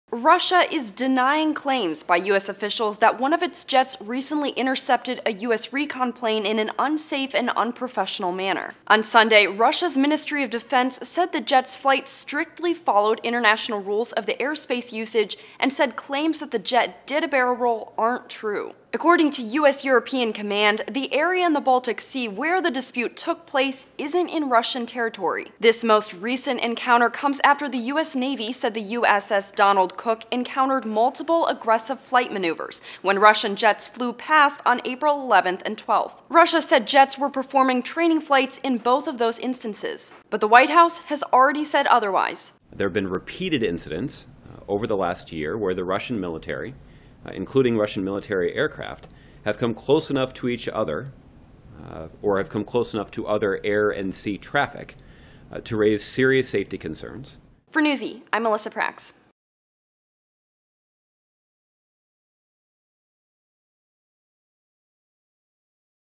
Russia Denies Claims That Its Jet Barrel-Rolled Over US Recon Plane - Newsy
A U.S. plane was said to have been intercepted by a Russian jet flying less than 50 feet away, however, the Russian Ministry of Defense denies these claims. According to the White House, this recent encounter is not the first sign of aggression within the last year.

Transcript:
Russia is denying claims by U.S. officials that one of its jets recently intercepted a U.S recon plane in an "unsafe and unprofessional manner."
On Sunday, Russia's Ministry of Defense said the jet's flight strictly followed "international rules of the airspace usage" and said claims that the jet did a barrel roll aren't true.
According to U.S. European Command, the area in the Baltic Sea where the dispute took place isn't in Russian territory.
This most recent encounter comes after the U.S. Navy said the USS Donald Cook "encountered multiple, aggressive flight maneuvers" when Russian jets flew past on April 11 and 12.
Russia said jets were performing training flights in both of those instances.
But the White House has already said otherwise.
"There have been repeated incidents over the last year where the Russian military, including Russian military aircraft, have come close enough to each other or have come close enough to other air and sea traffic to raise serious safety concerns," White House Press Secretary Josh Earnest said.
This video includes clips from U.S. Navy, the White House and images from U.S. Air Force, Dmitriy Pichugin and U.S. Navy.